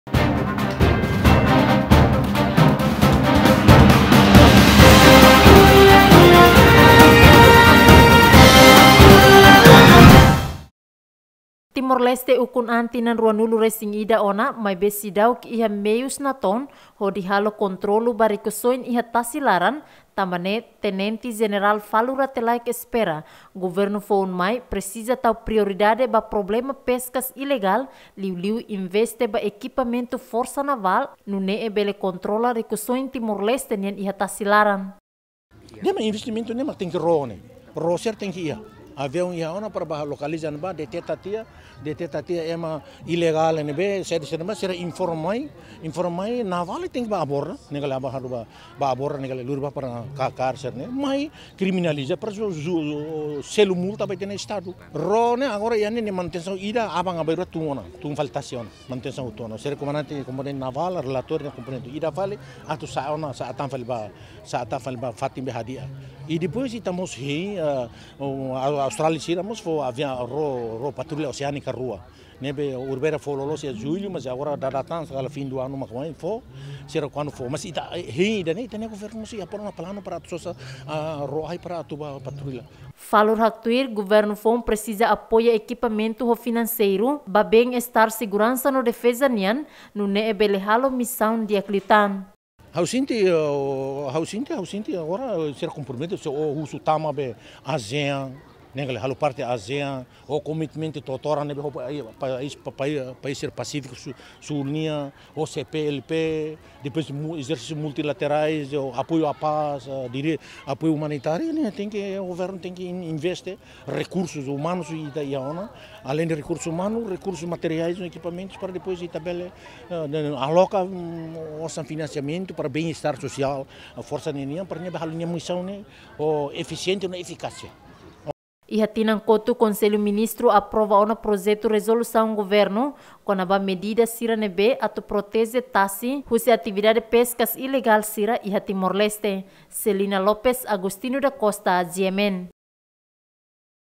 Tenente Jenerál husu Governu foun, halo investimentu ba Forsa Navál hodi kontrola rikusoin iha tasi
Tenente Jenerál Falur Rate Laek husu Governu Da Sia halo investimentu ba Forsa Navál hodi kontrola rikusoin iha tasi